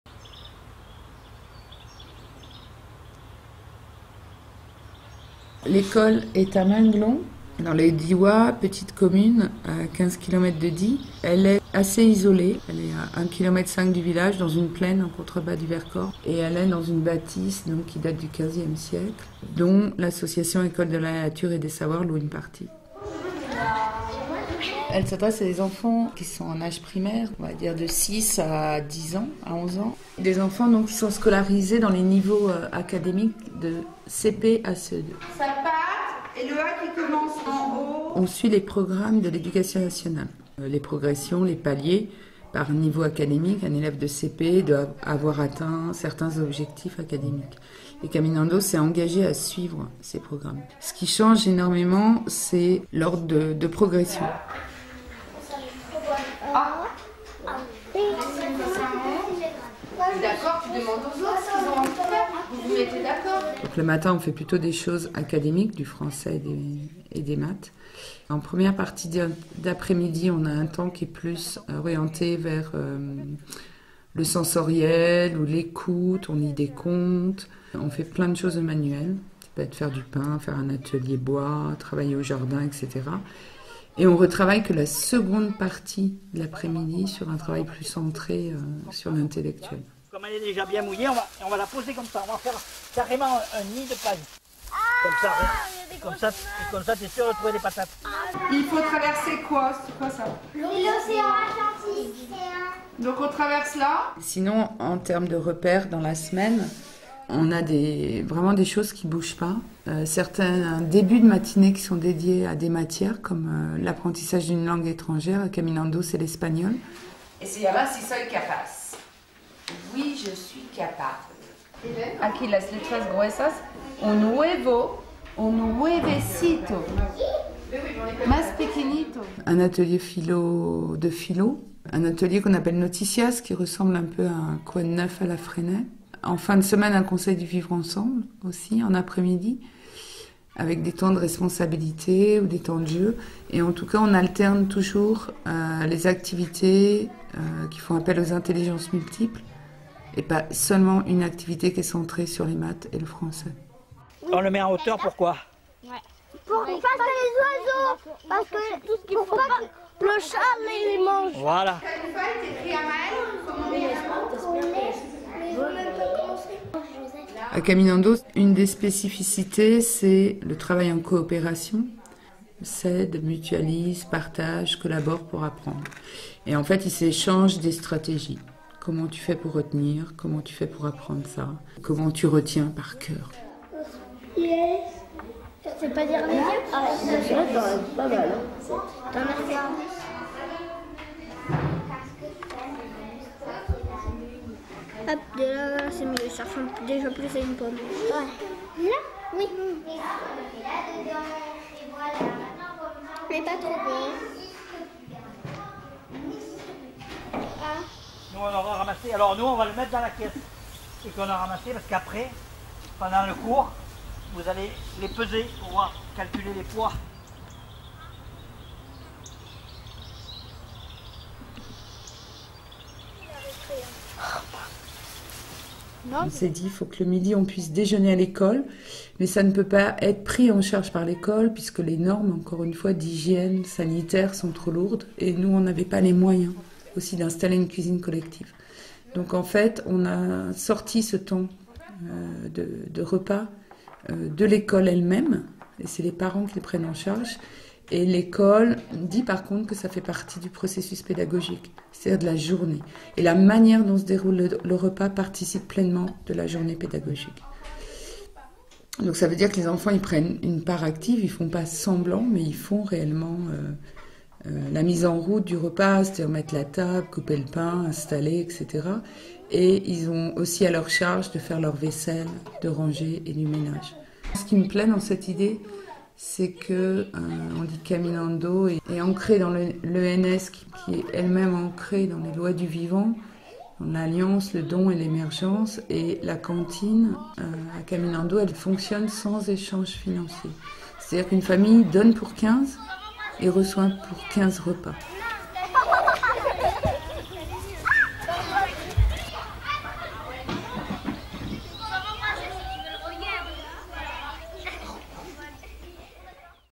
L’école Caminando (Drôme), une école qui respire
En pleine nature, dans le Diois (Drôme), à l'école Caminando, les enfants apprennent à coopérer, à communiquer et à jardiner au même titre qu'à progresser en mathématiques et en français. Ou comment prendre le temps de nourrir tous les talents sans distinction.

Pour aller plus loin, lire l'article "une école qui respire" publié dans Kaizen 19